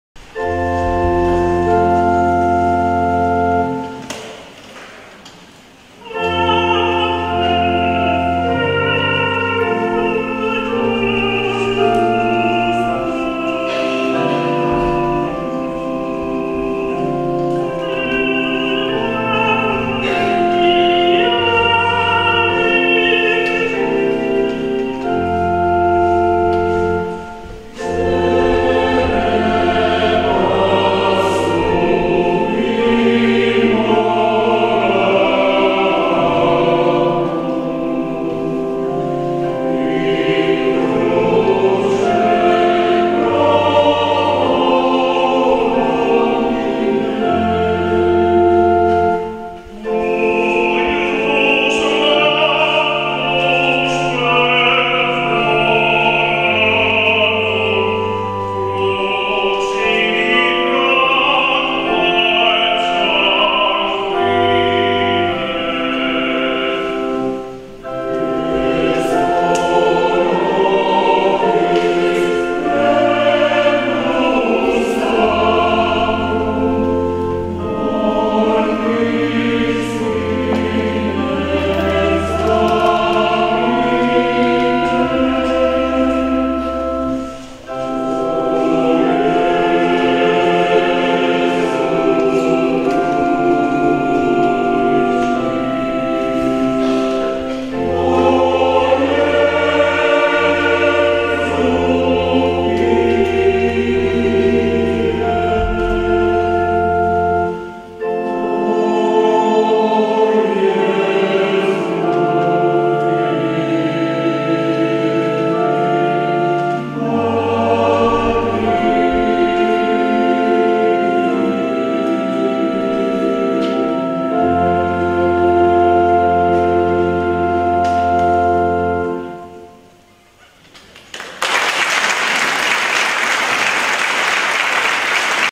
GRADISCA SAN VALERIANO - ARMONIE IN FORTEZZA - CORO HAELIETUM DI ISOLA D'ISTRIA (1)
GRADISCA D'ISONZO - CHIESA DI SAN VALERIANO - 20 MAGGIO 2017 - XI RASSEGNA CORALE ARMONIE IN FORTEZZA  - CORO HALIAETUM DI ISOLA D'ISTRIA - SLO